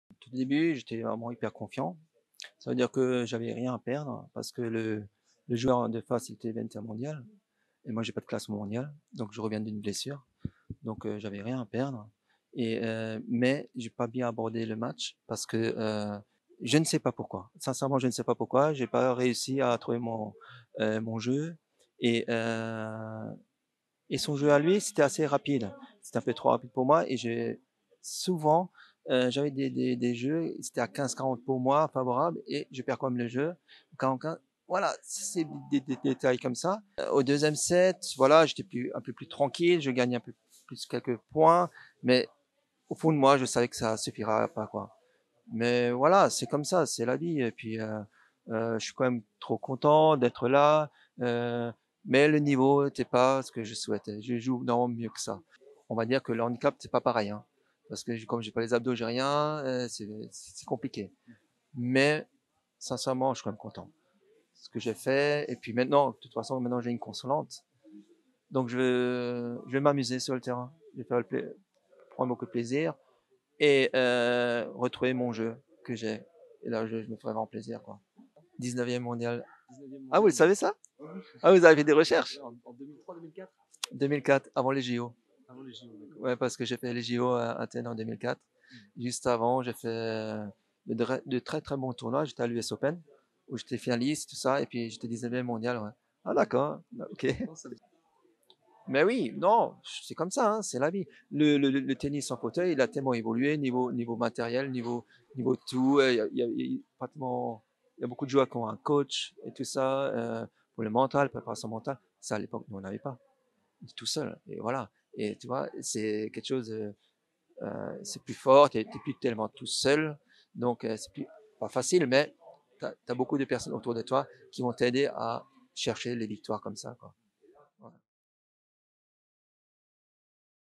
Laurent Fischer : « Le tennis-fauteuil a tellement évolué en quinze ans »
Ancien numéro 1 français entre 2002 et 2004, Laurent Fischer a été éliminé en 1/8e de finale de l’Open de Paratennis du Loiret. Sur le score de 0-6 3-6, il est battu par le jeune Autrichien Nico Langmann, 21e au ranking. Lui-même ancien membre du top 20 mondial, le Français est désormais une figure expérimentée du circuit du tennis-fauteuil, lui qui a traversé les années dans la discipline.